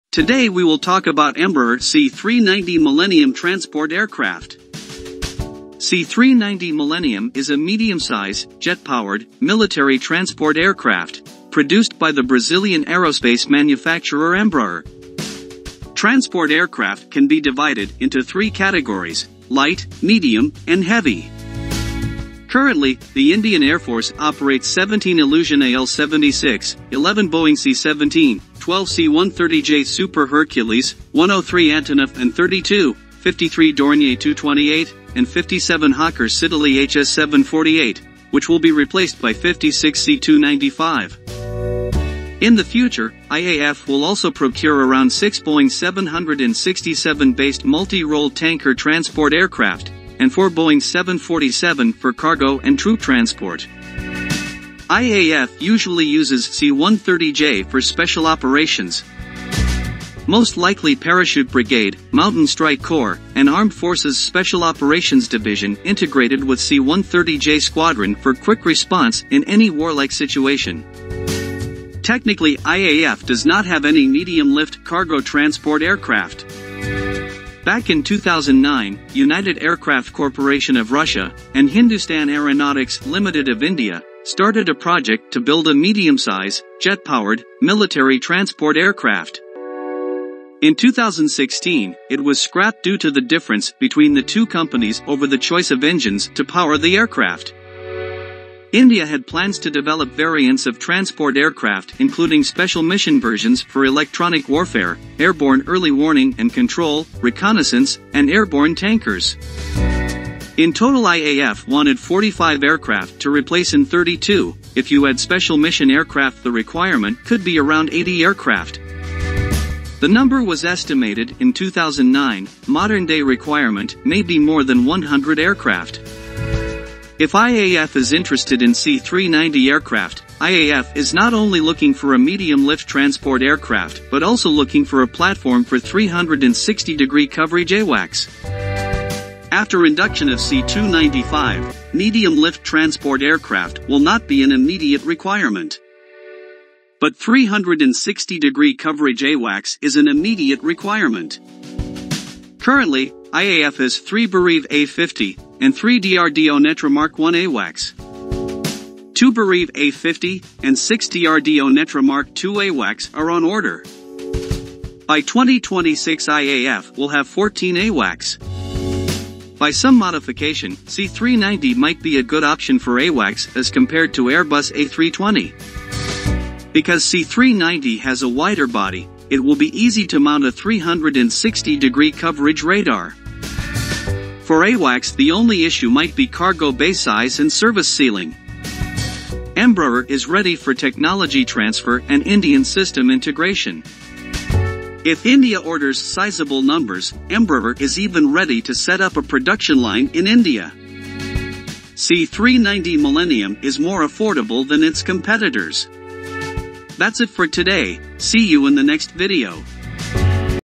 Embraer C-390 Millennium For Indian Airforce ? | Analysis | in English
Embraer C-390 Millennium For Indian Airforce ? | Analysis.

Today we will talk about Embraer C-390 Millennium transport aircraft. C-390 Millennium is a medium-size, jet-powered military transport aircraft produced by the Brazilian aerospace manufacturer Embraer. Transport aircraft can be divided into 3 categories: light, medium, and heavy. Currently, the Indian airforce operates 17 Ilyushin Il76, 11 Boeing C17, 12 C130J Super Hercules, 103 Antonov An32, 53 Dornier 228, and 57 Hawker Siddeley HS748, which will be replaced by 56 C295. In the future, IAF will also procure around 6 Boeing 767-based Multi Role Tanker Transport aircraft and 4 Boeing 747 for cargo and troop transport. IAF usually uses C130J for special operations. Most likely parachute brigade, mountain strike corps, and Armed Forces Special Operations Division integrated with C130J Squadron for quick response in any war-like situation. Technically IAF does not have any medium lift cargo transport aircraft.